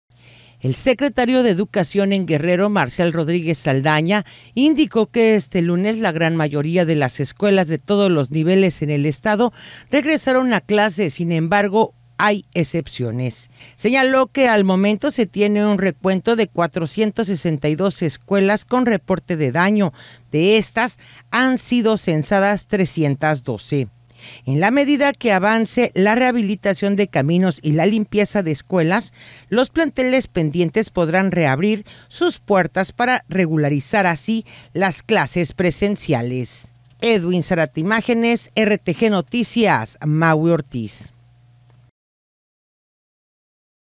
RTGnoticias | Hay excepciones para regreso a clases, aclara la SEG
El secretario de Educación en Guerrero, Marcial Rodríguez Saldaña, indicó que este lunes la gran mayoría de las escuelas de todos los niveles en el estado regresaron a clases; sin embargo, hubo excepciones.